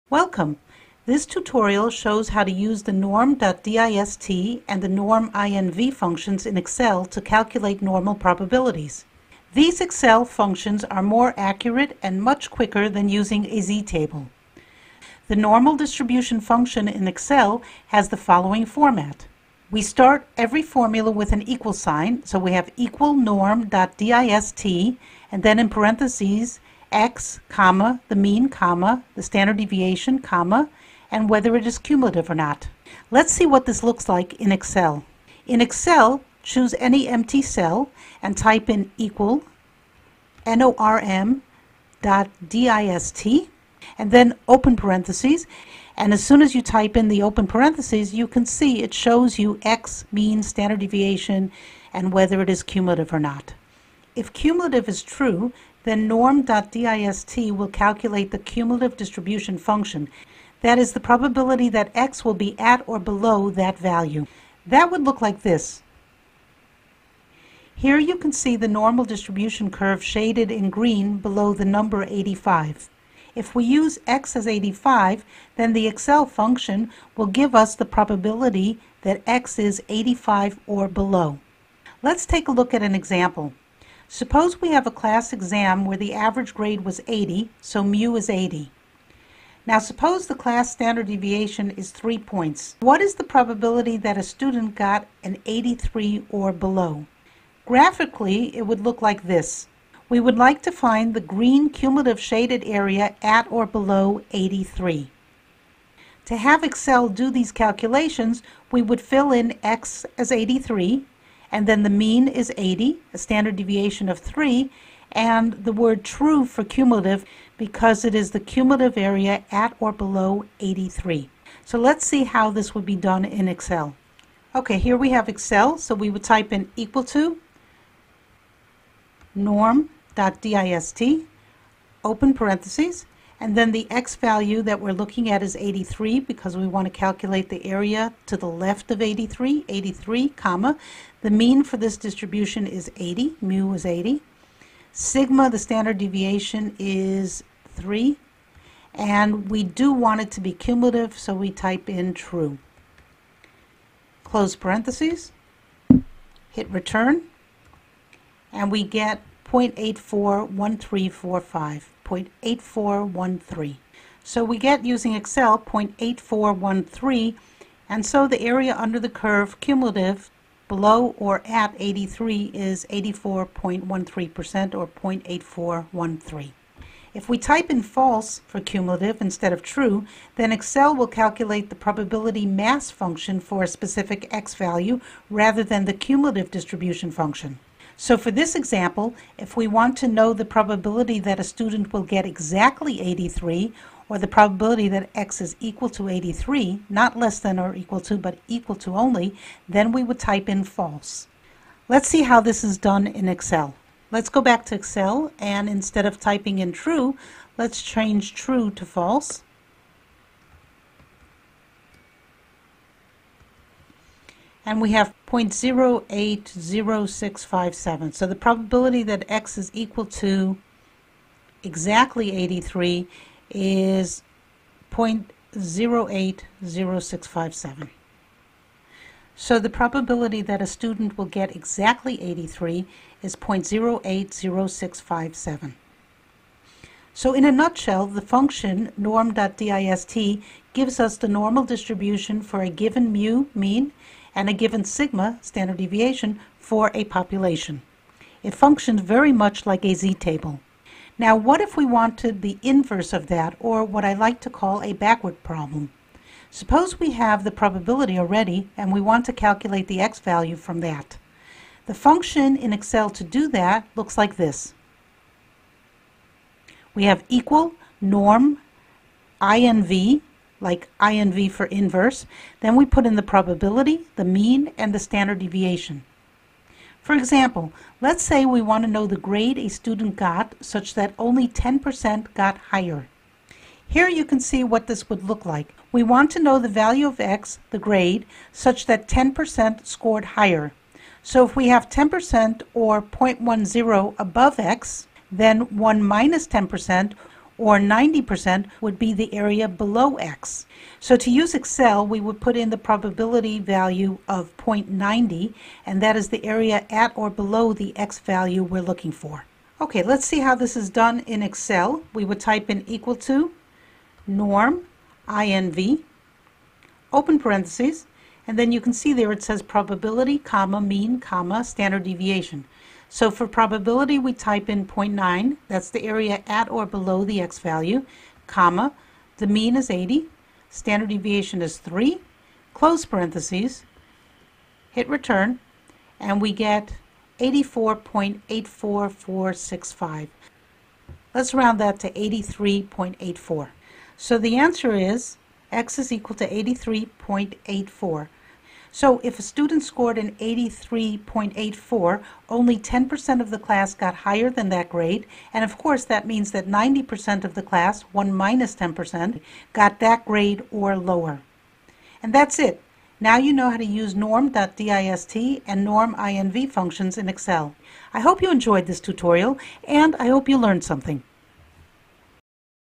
How to Use Excel's Normal Distribution Function =NORM.DIST and =NORM.INV
This tutorial demonstrates how to use =NORM.DIST, and =NORM.INV in Excel.   Please note, older versions of Excel used =NORMINV without the dot, current versions use =NORM.INV.  These functions are much quicker and easier to use than a Z table, and of course the results are more accurate.  This video uses an example to show how the functions are calculated.  For a more in-depth review please view my tutorial on the Normal Distribution and Using the Z Table at: